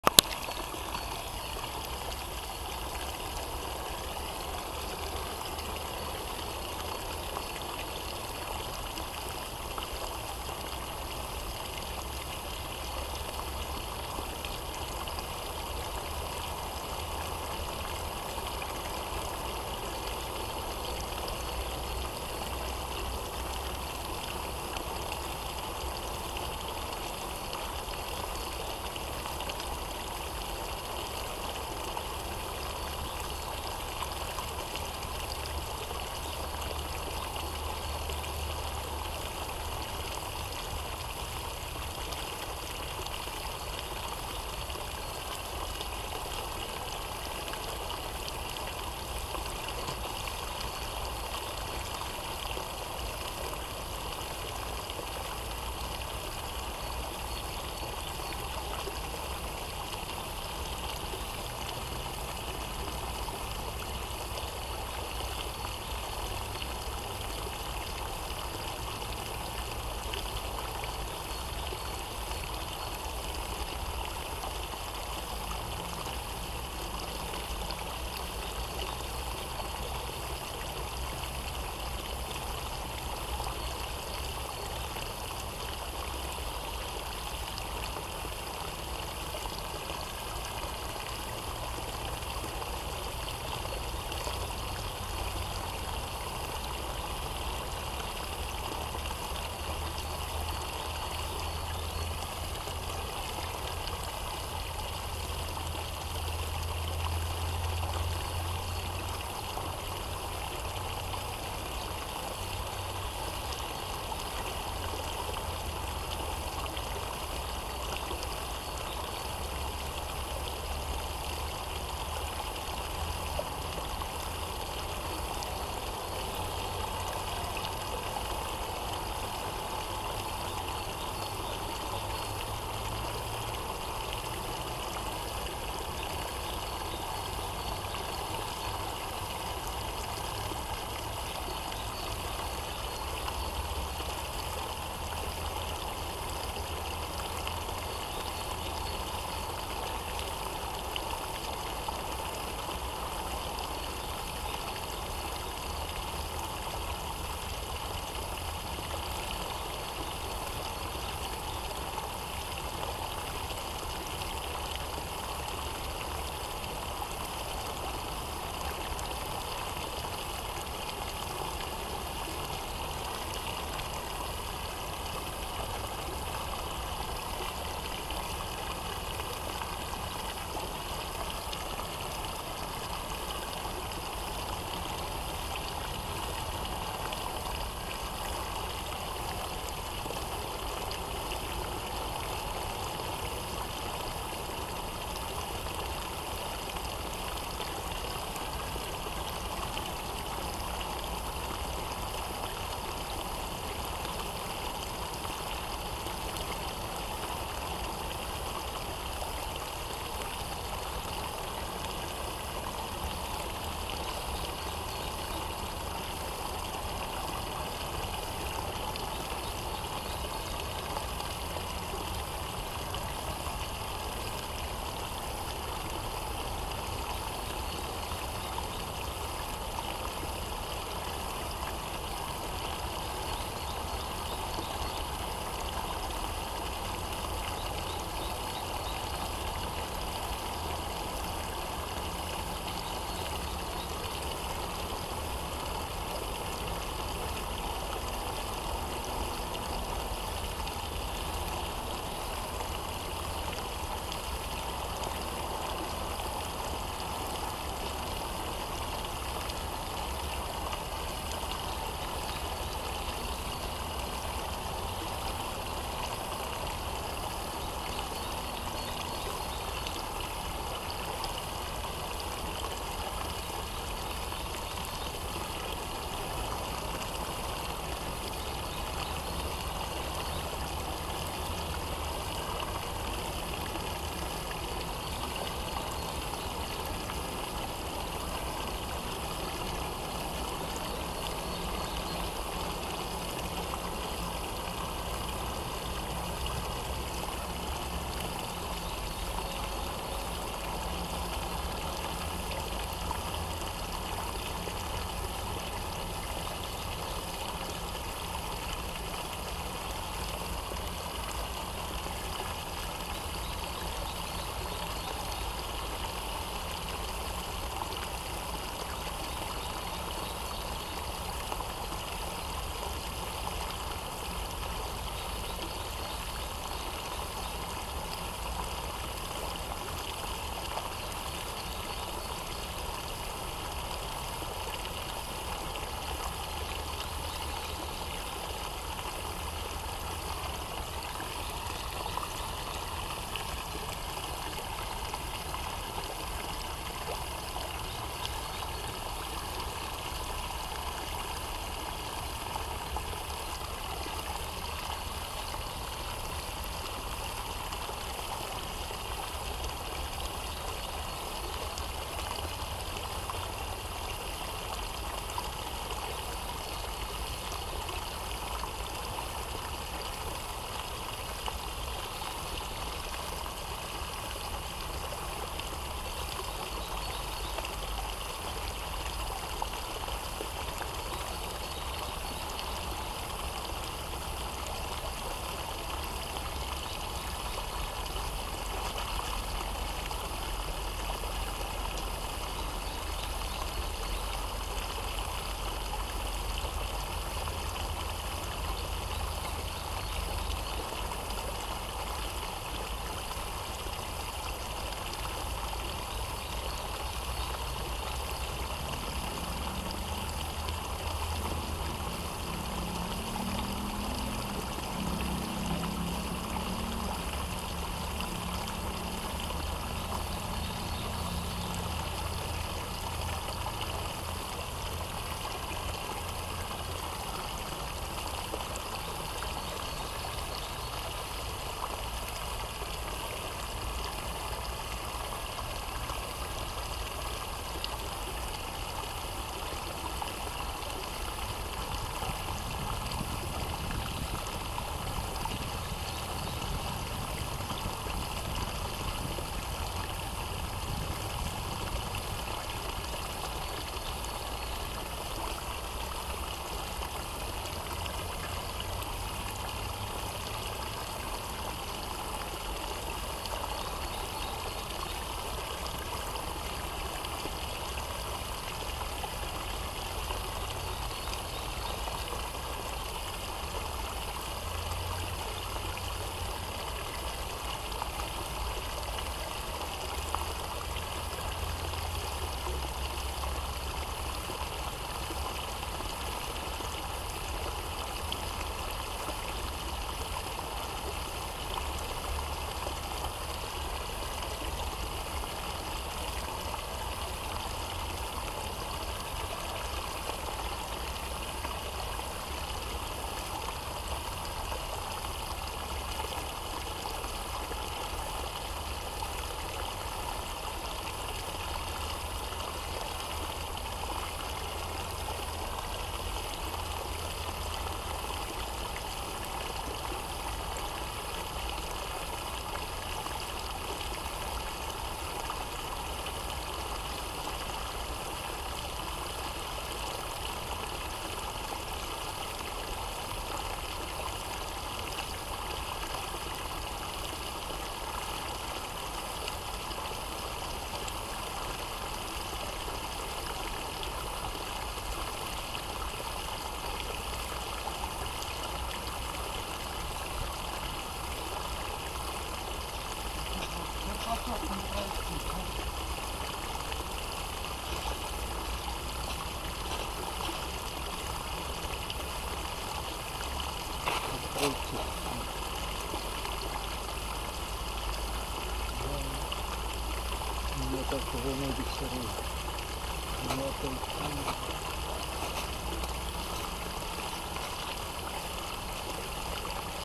Ручеёк (общий план)
Пятница, 13 апреля 2012. Нашёл ручеёк в местном лесопарке. Поют птички и журчит вода. Красота!